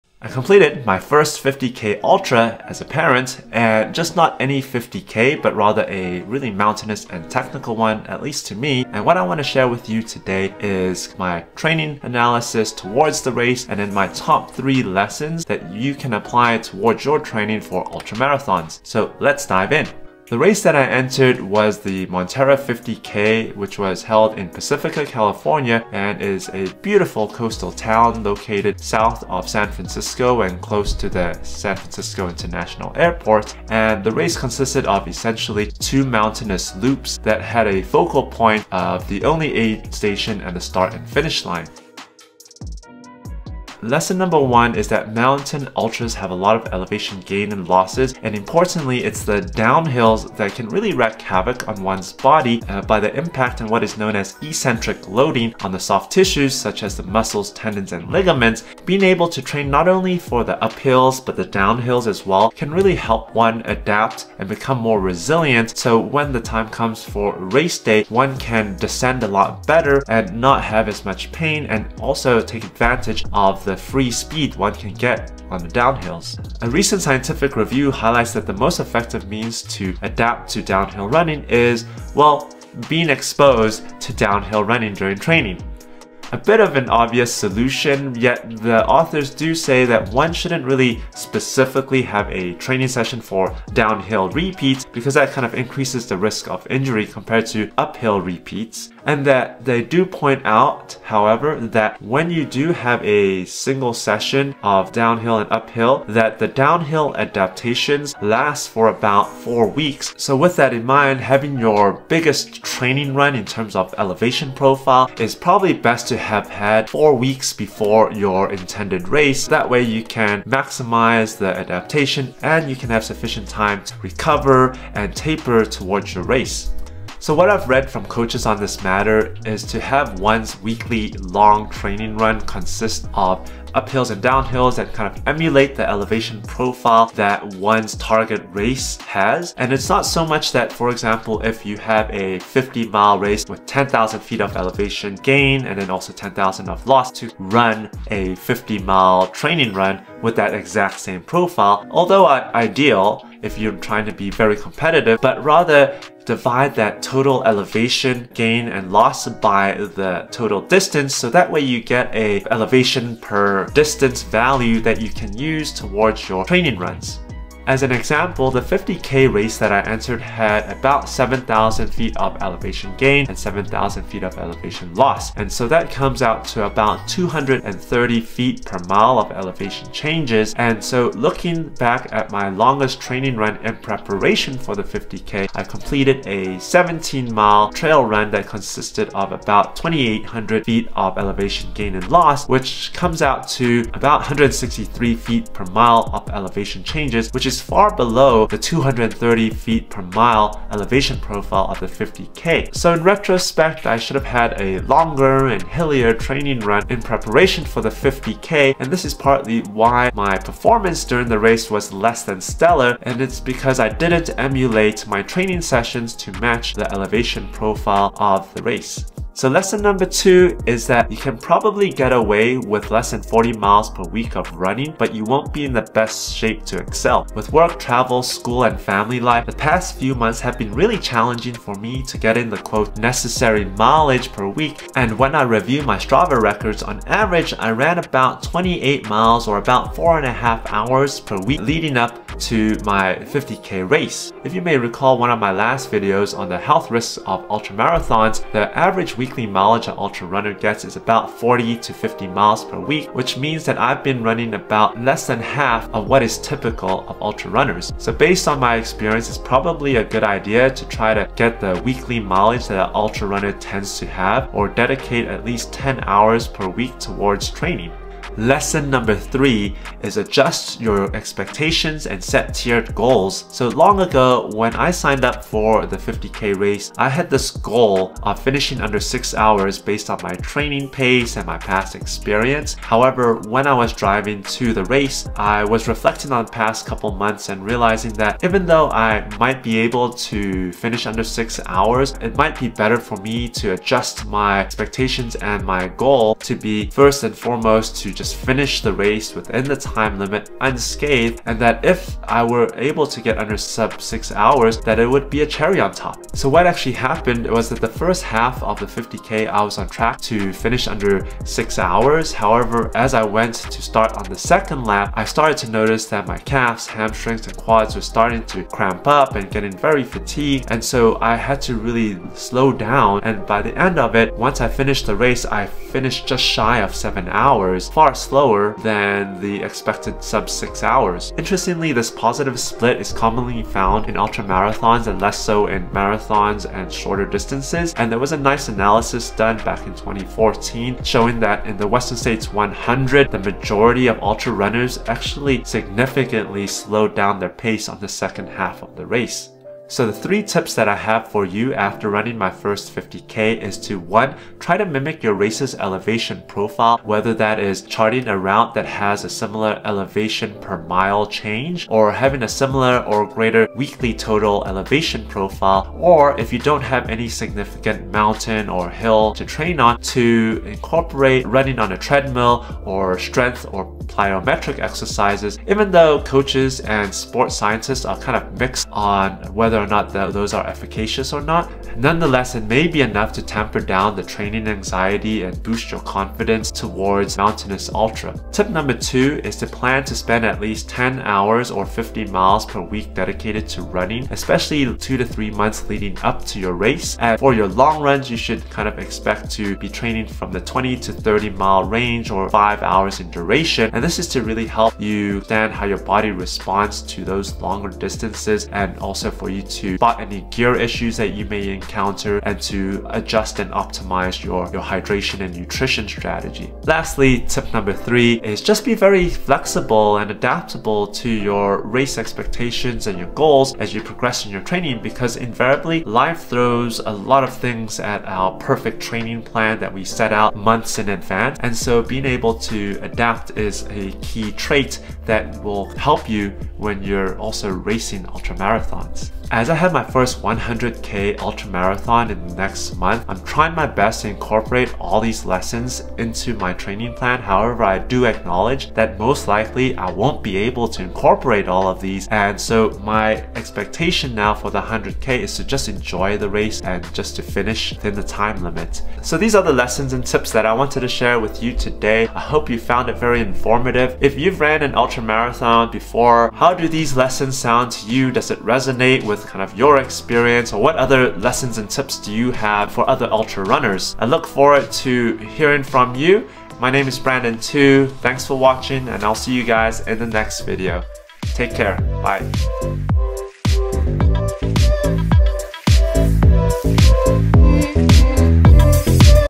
Three Training Tips for Surviving an Ultramarathon | Lessons from A 50K Race
Lessons from running a mountainous 50K ultramarathon in Pacifica, CA. An update on my ultra journey as a dad.

Bontemps B, Vercruyssen F, Gruet M, Louis J. Downhill Running: What Are The Effects and How Can We Adapt? A Narrative Review. Sports Med. 2020 Dec;50(12):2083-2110.